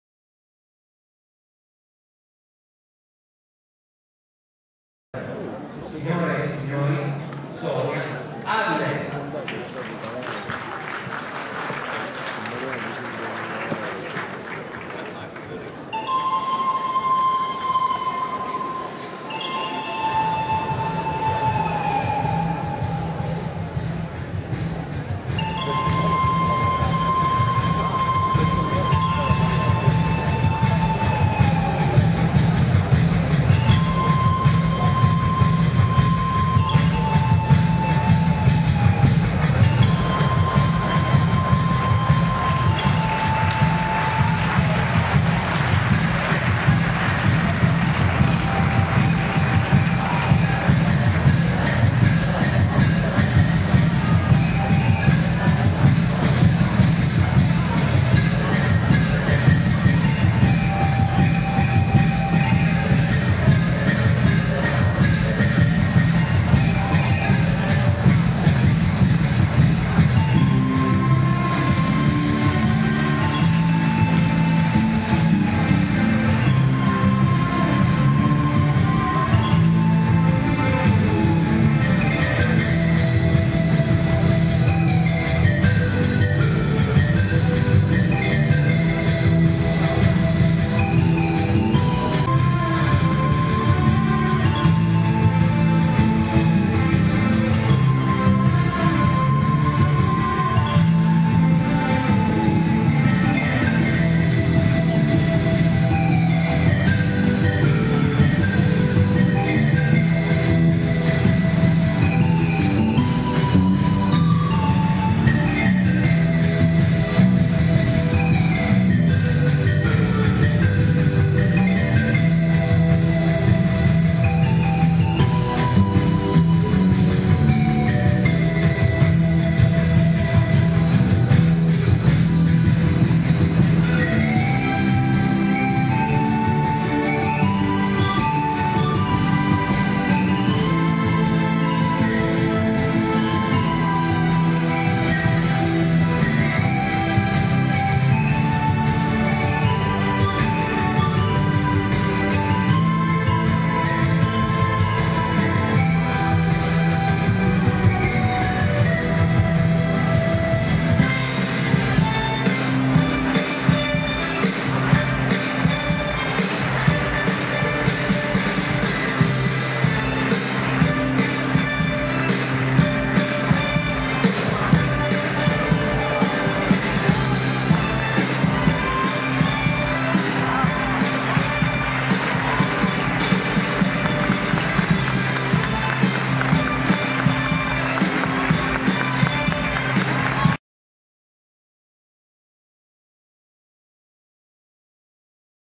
Arte Mujer Bartolina a MedModa - Caltanissetta Sicilia - Italia.wmv
25 marzo 2010  15^ edizione  MEDMODA  al teatro  REGINA MARGHERITA  A CALTANISSETTA  SICILIA - ITALIA